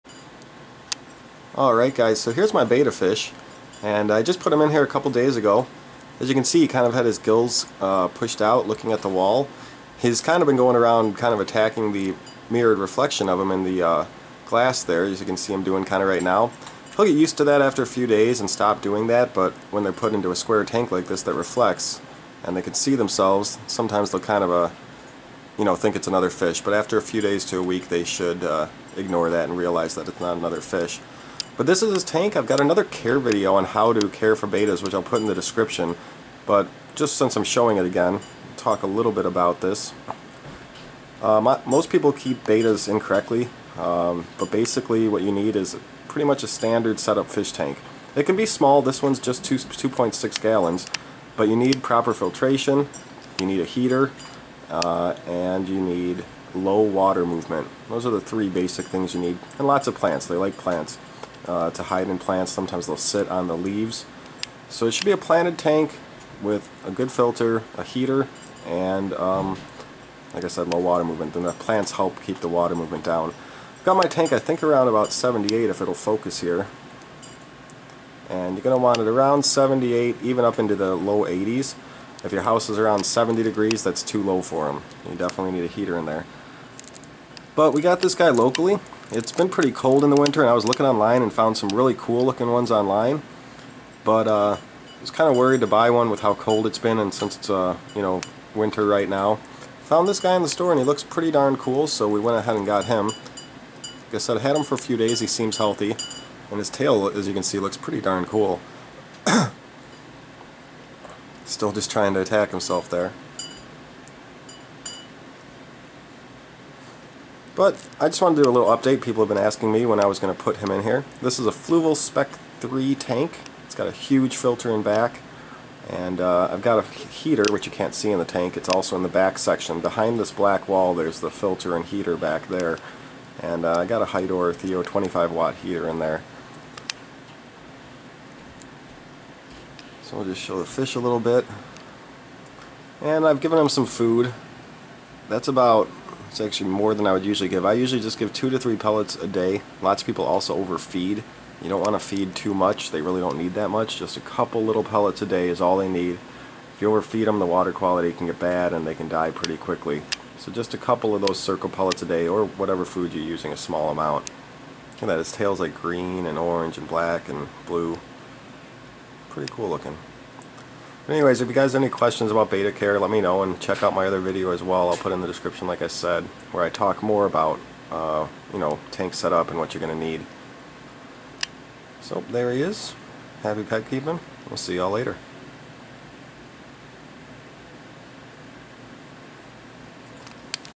Betta Fish Tank Setup - Added Our Betta
Showing the new Betta fish and discussing Betta care a little bit.  For more information on Betta fish tank setup, see my care video below.